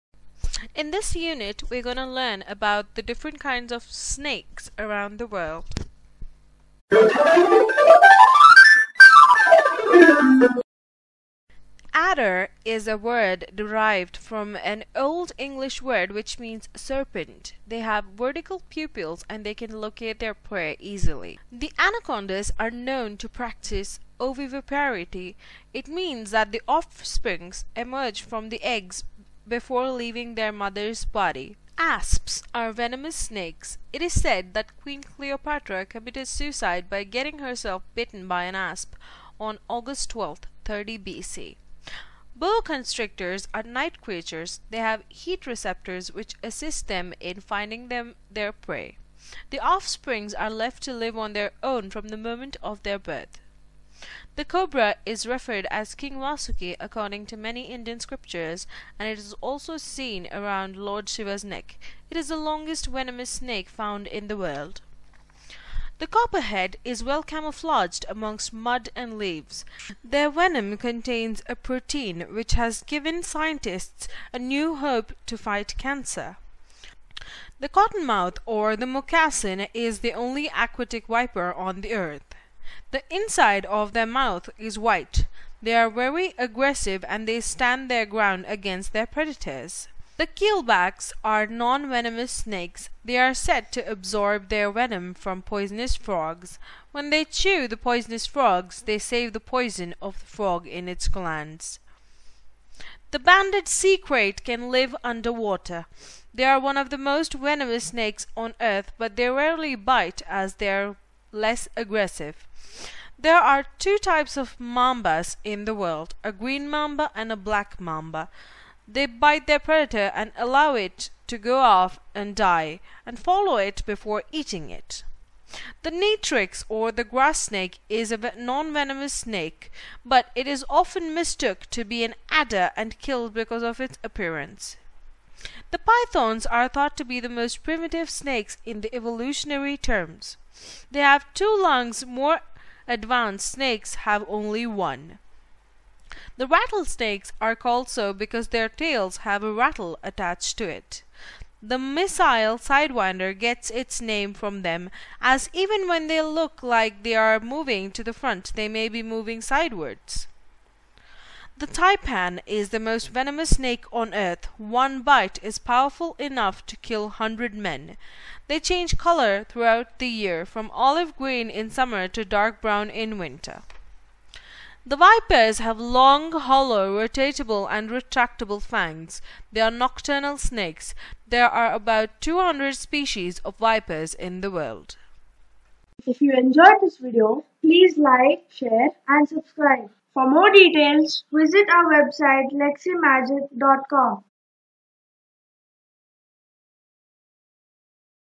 Types of Snakes
Snakes – the legless wonders of nature – have always fascinated us.  Review the names of 17 types of snakes.

Words Reviewed:
Adder, Anaconda, Asp, Boa, Cobra, Copperhead, Cottonmouth, Moccasin, Keelback, Krait, Mamba, Natrix, Python, Rattlesnake, Taipan, Sidewinder, Viper